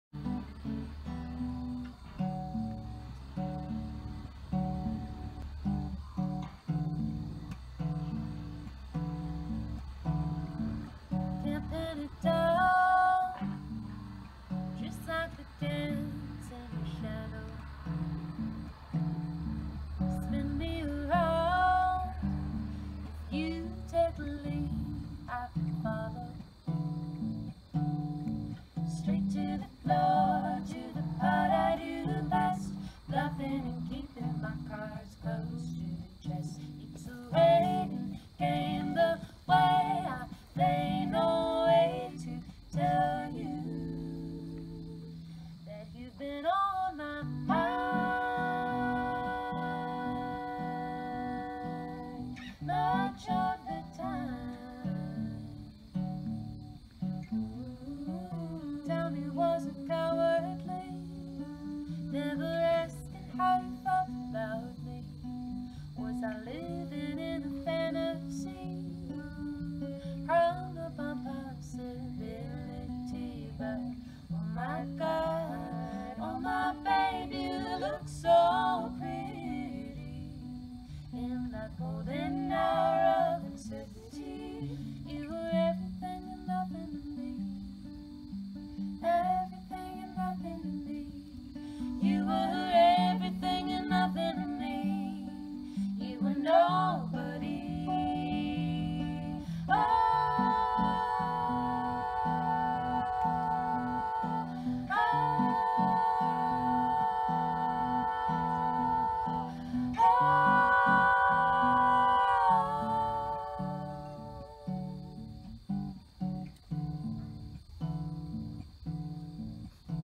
The sweet harmonies of The Ladles on a lazy, hazy Sunday afternoon in Little Valley, NY
Performing at Native Offerings Farm.